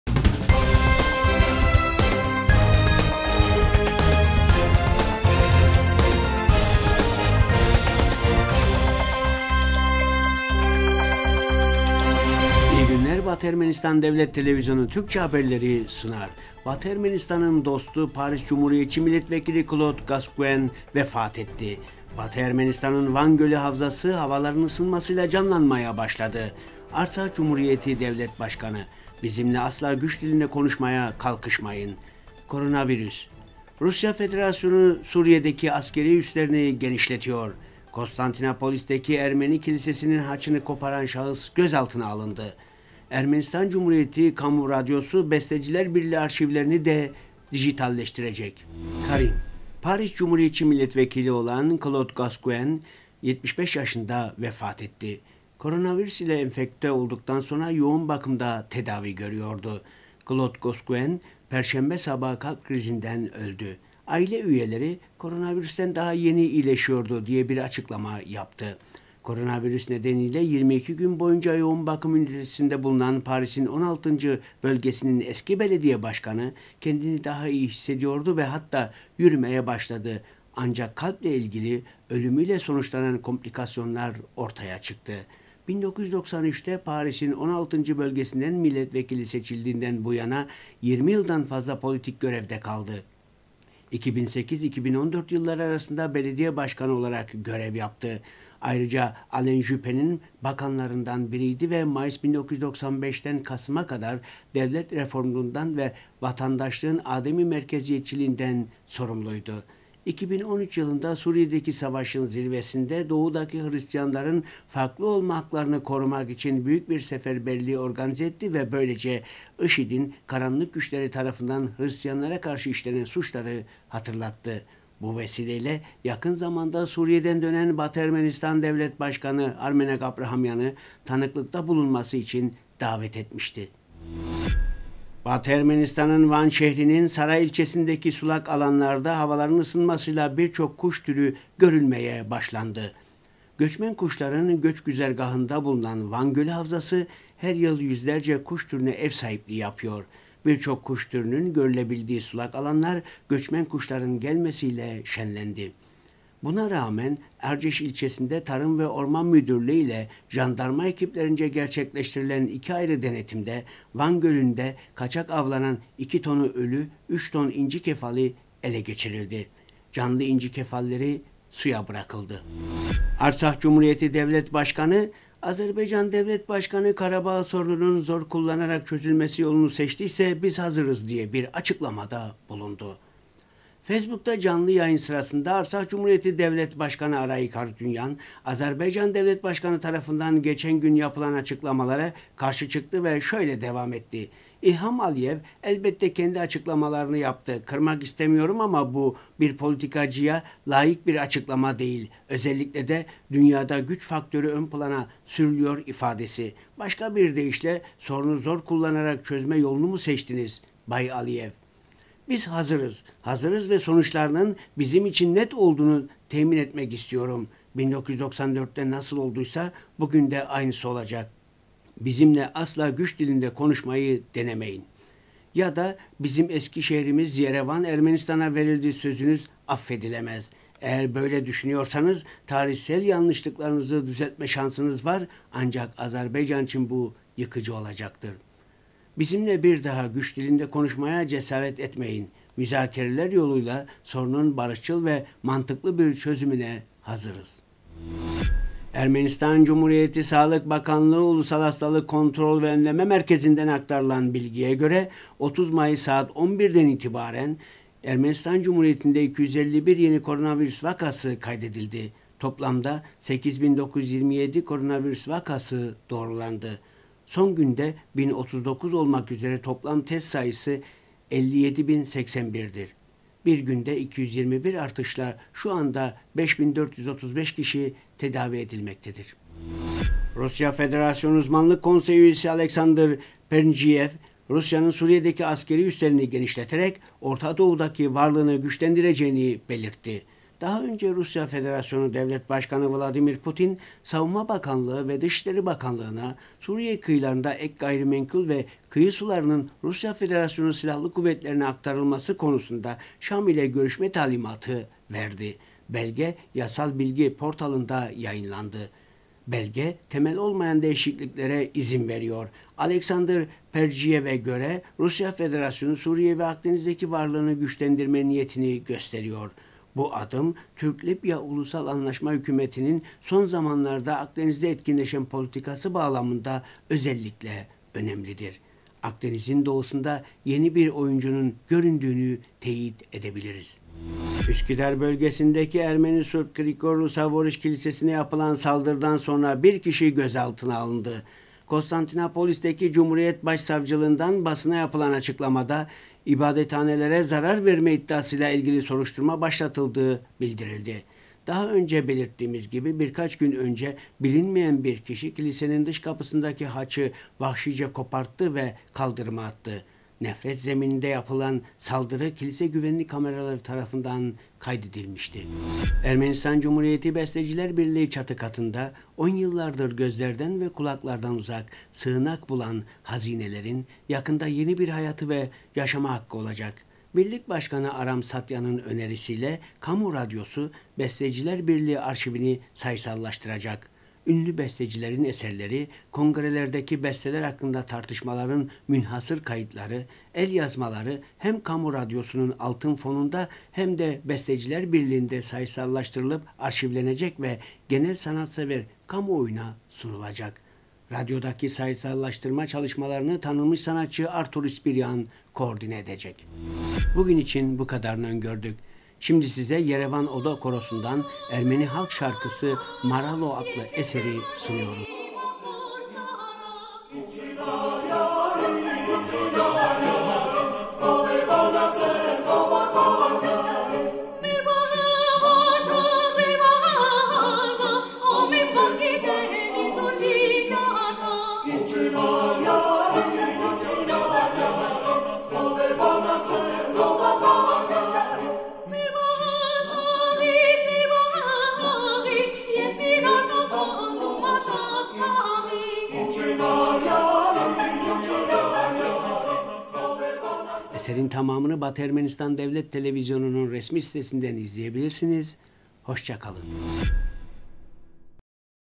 Rusya Federasyonu Suriye'deki askeri üslerini genişletiyor.Batı Ermenistan haberleri 30-05-2020
1- Batı Ermenistan'ın dostu, Paris Cumhuriyetçi Milletvekili Claude Gasguen vefat etti.
2- Batı Ermenistan’ın Van Gölü havzası havaların ısınmasıyla canlanmaya başladı.
3- Artsakh Cumhuriyeti Devlet Başkanı: "Bizimle asla güç dilinde konuşmaya kalkışmayın."
4- Koronavirüs.
5- Rusya Federasyonu Suriye'deki askeri üslerini genişletiyor.
6- Konstantinopolis'teki Ermeni Kilisesi’nin haçını koparan şahıs gözaltına alındı.
7- Ermenistan Cumhuriyeti Kamu Radyosu, Besteciler Birliği arşivlerini de dijitalleştirecek.